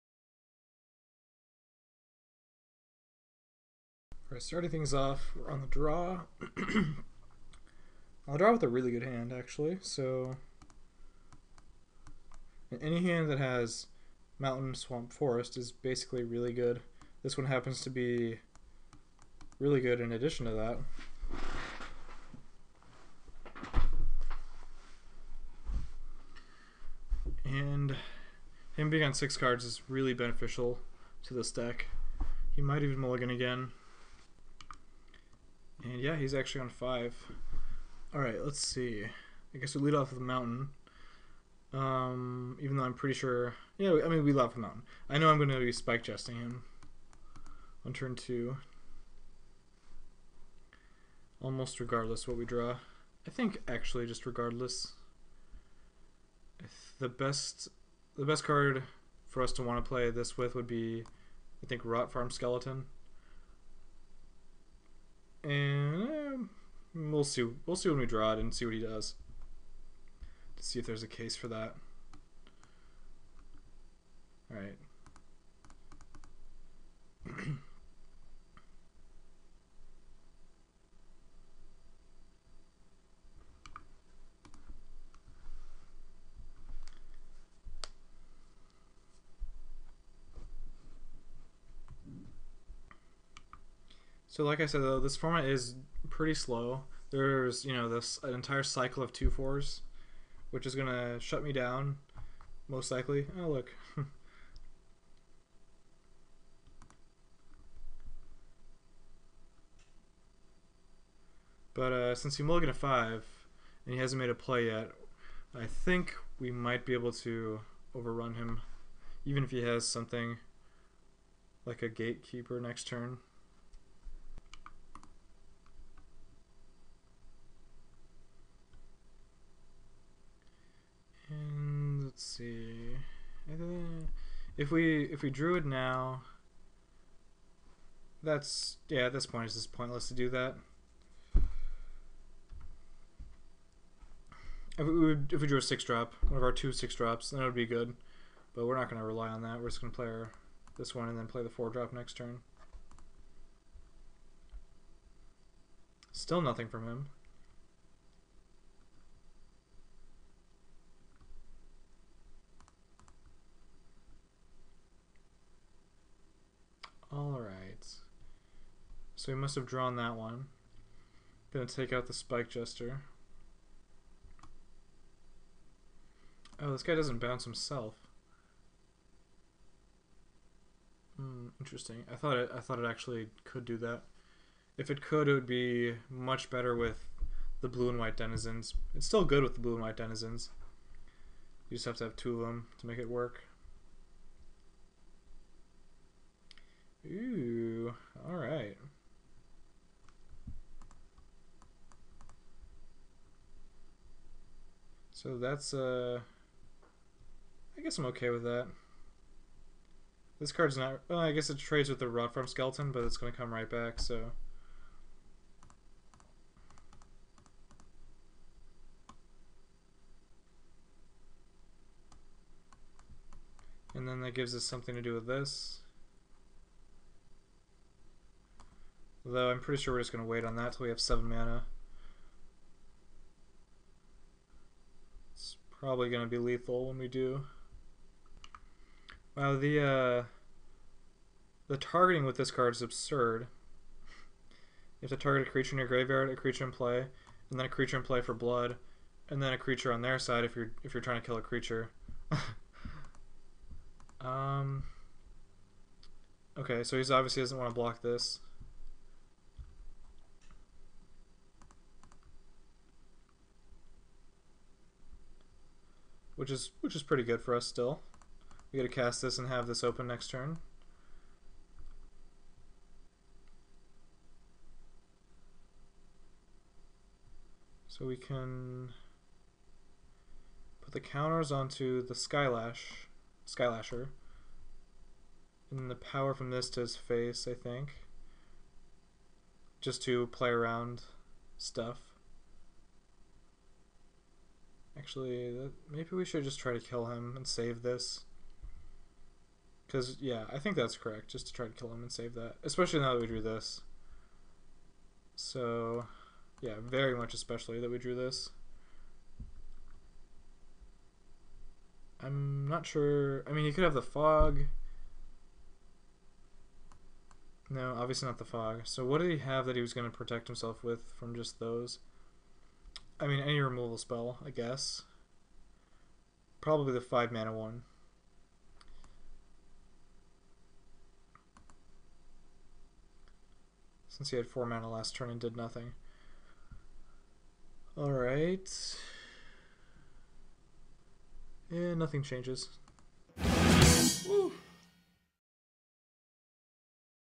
Channel _ShipItHolla - DDD Draft #1 (Match 1, Game 1)
Michael Hetrick navigates a triple-Dragon's Maze draft in the prerelease!